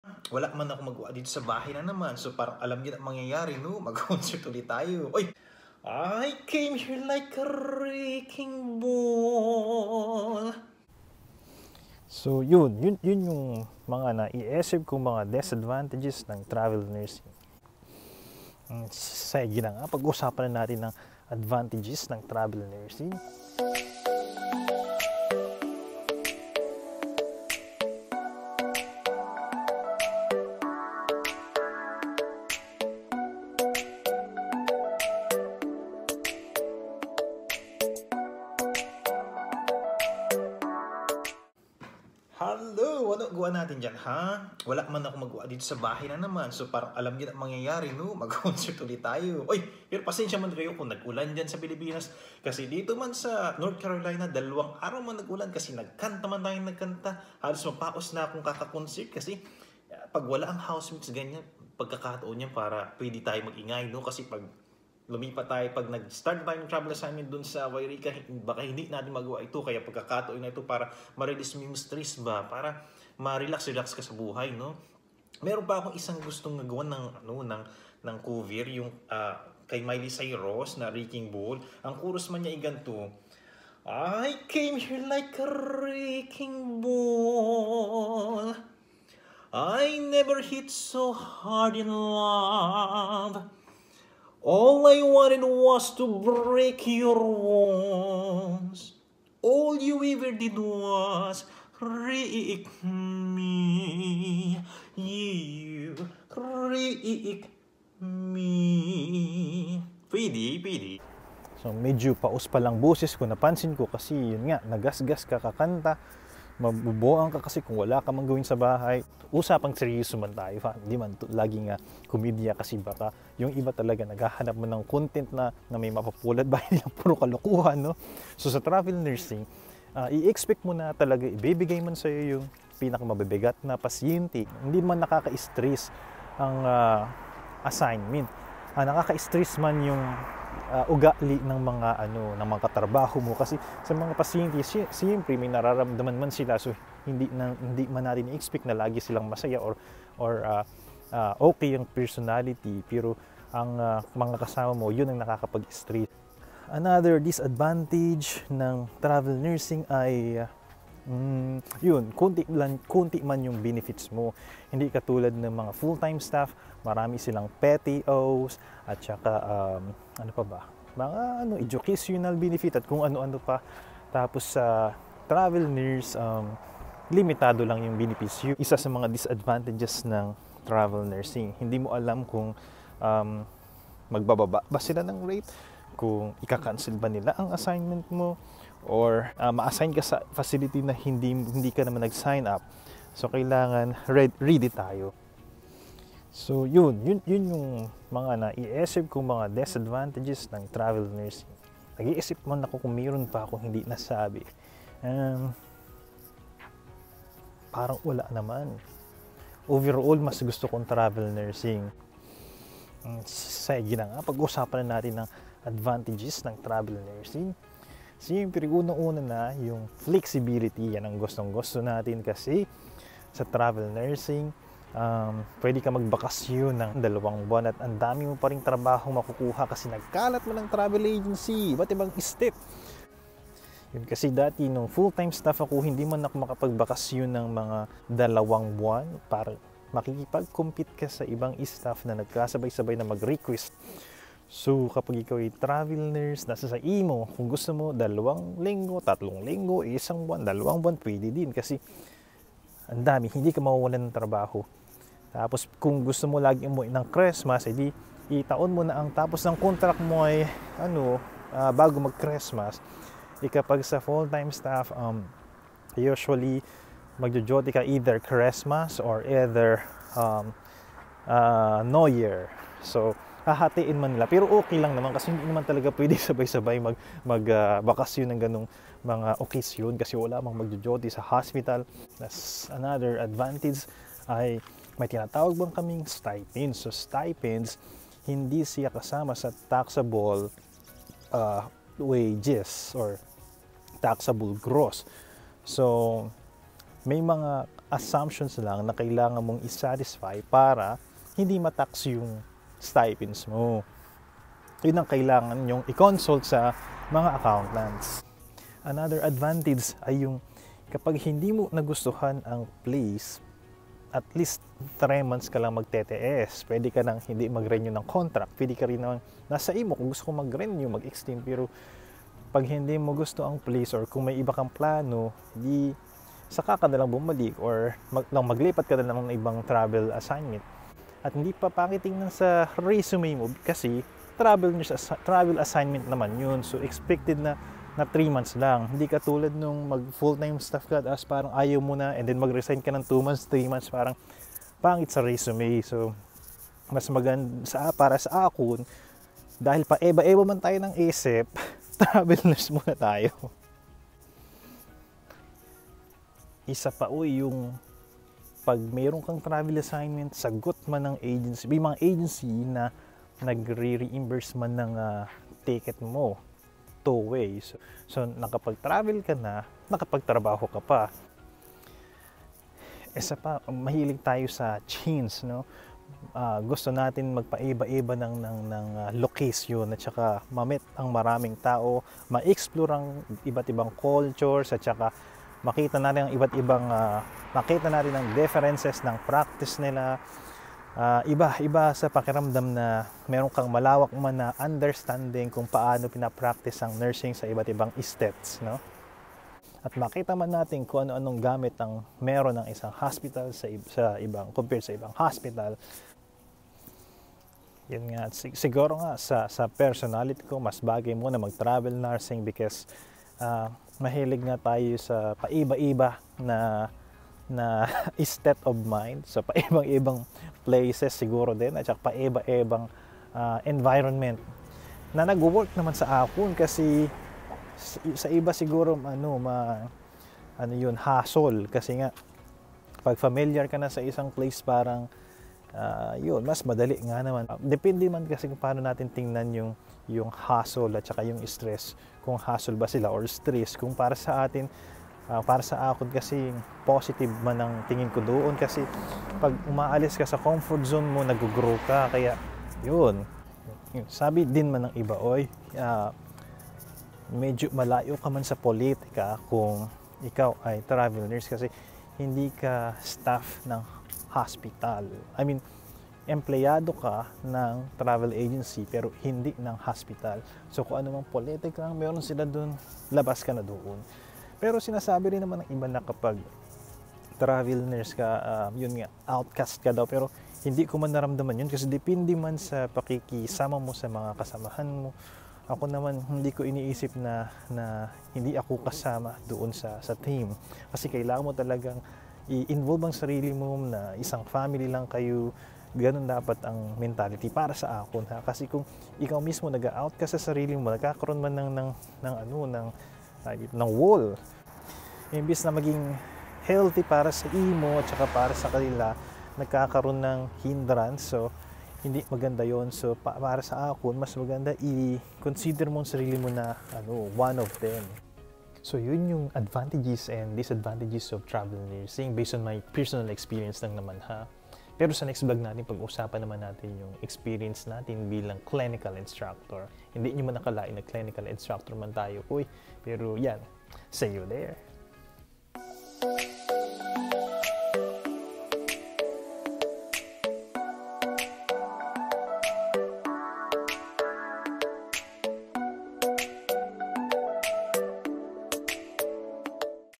Pros & Cons of Travel Nursing: Personal Experience & Perspectives
There is a lot to discuss about travel nursing. This video is just an overview of the advantages and disadvantages of travel nursing career.

NoypiNurseJhune